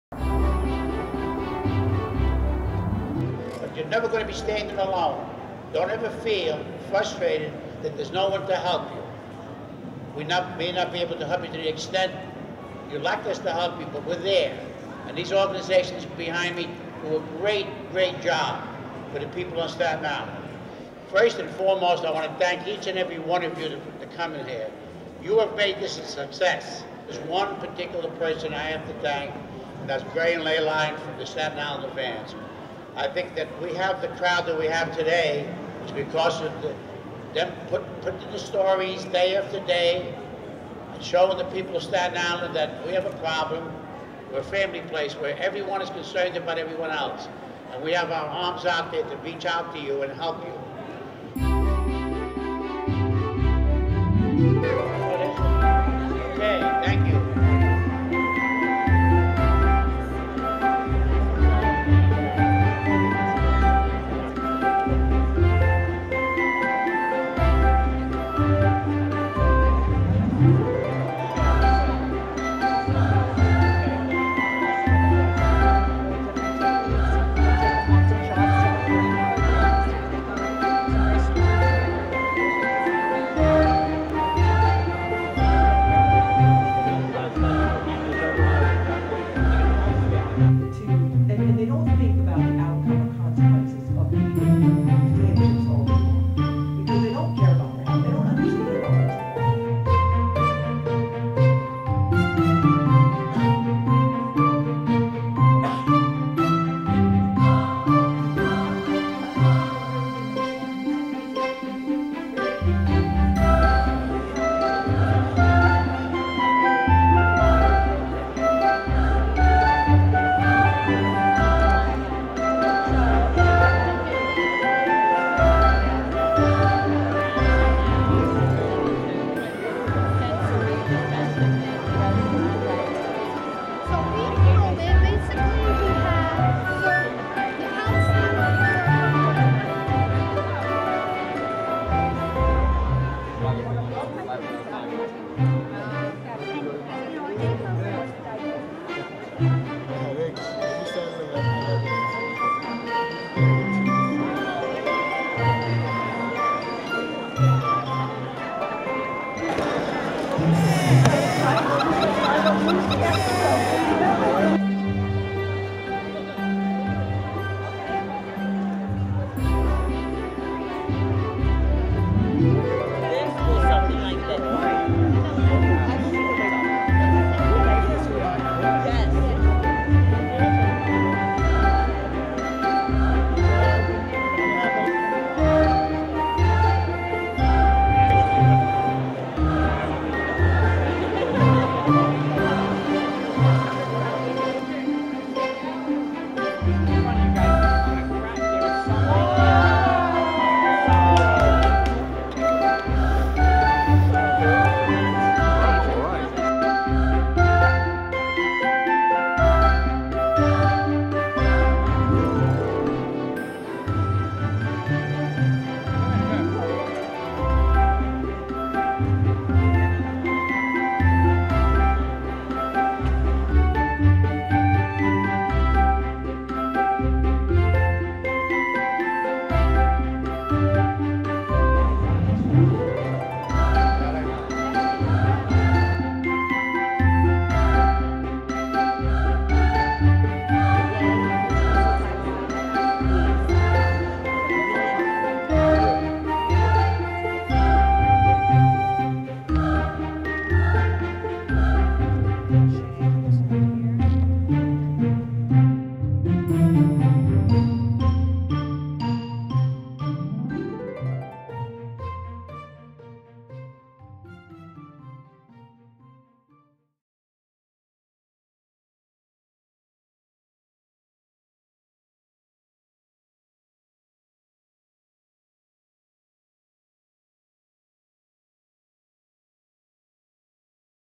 Over 600 Come Out for Autism Resource Fair on Staten Island
Over 600 Staten Islanders joined with Borough President James P. Molinaro as he held an Autism Resource Fair to present vital information to parents and caregivers of autistic children and young adults.  Service providers were on hand to provide families with information and assistance, and seminars were held detailing important advancements in technology that can assist people on the autism spectrum.